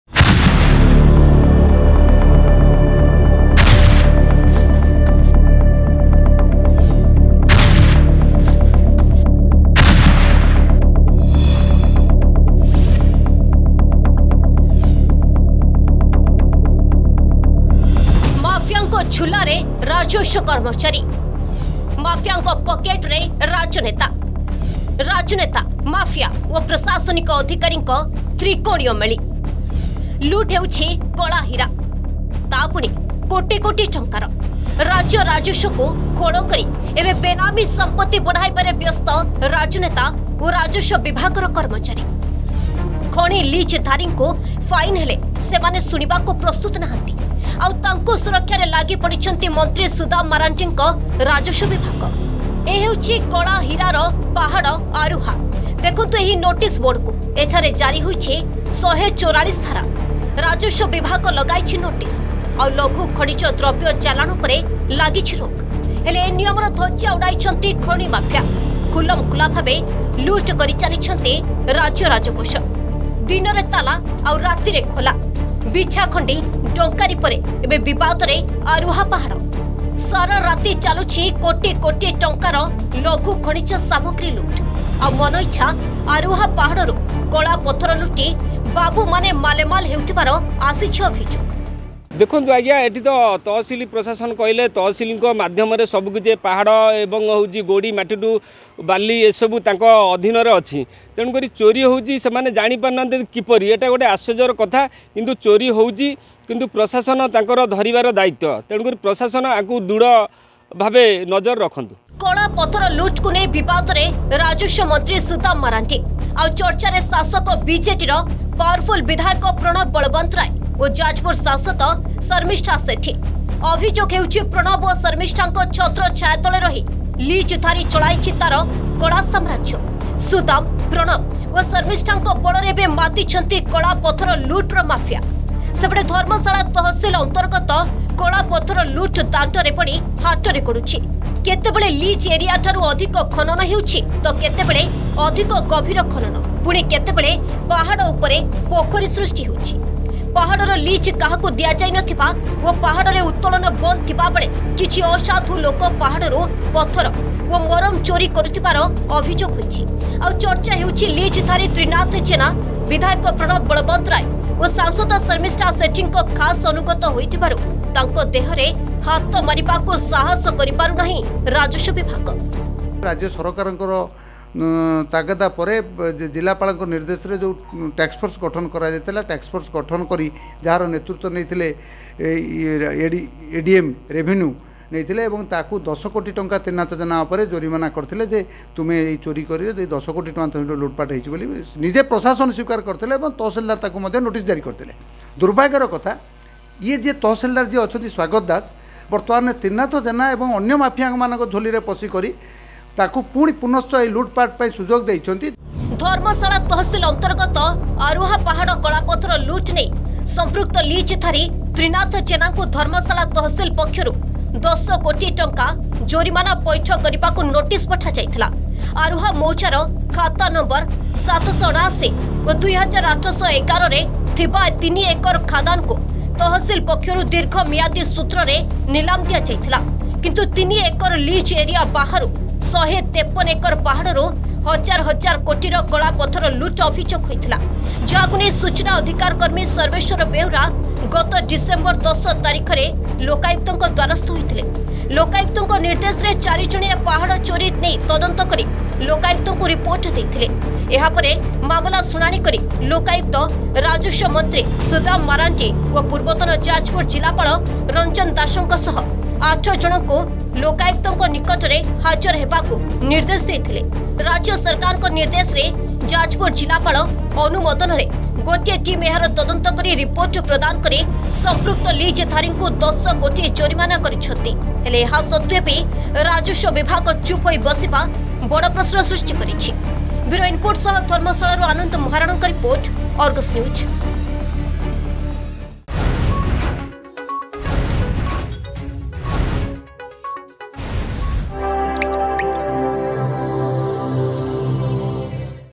Black Diamond is Being Stolen by Ministers, Officials, and the Mafia in Jajpur
Black Diamond is Being Stolen by Ministers, Officials, and the Mafia
କଳାପଥର ଲୁଟର ଭୂସ୍ବର୍ଗ ପାଲଟିଛି ଯାଜପୁରର ଧର୍ମଶାଳା । ଡଙ୍କାରୀ, ବିଛାଖଣ୍ଡି ମହାଲୁଟ୍ ପରେ ଏବେ ଚର୍ଚ୍ଚାରେ ଆରୁହା ପାହାଡ । କଳାହୀରାର ପାହାଡ ଆରୁହାରୁ ଲୁଟ୍ ହେଉଛି ହଜାର ହଜାର କୋଟିର ପଥର ଓ ମୋରମ । ସବୁ ଦେଖି ବି ଚୁପ୍ ବସୁଛି ରାଜସ୍ବ ବିଭାଗ